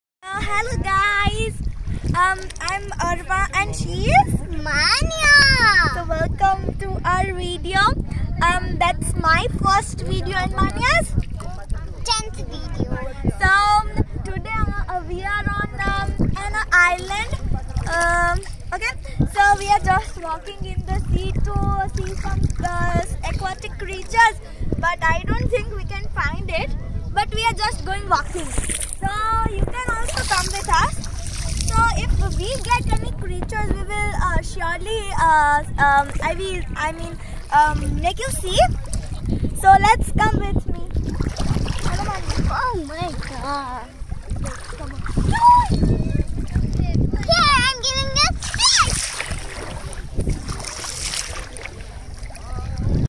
Visit to Pirotan Island Jamnagar- 1
Hiii guys,its Maanya here today I visited Pirotan island, Jamnagar. I enjoyed so much and saw many creatures with my friend arva.Hope you all also enjoy watching the video.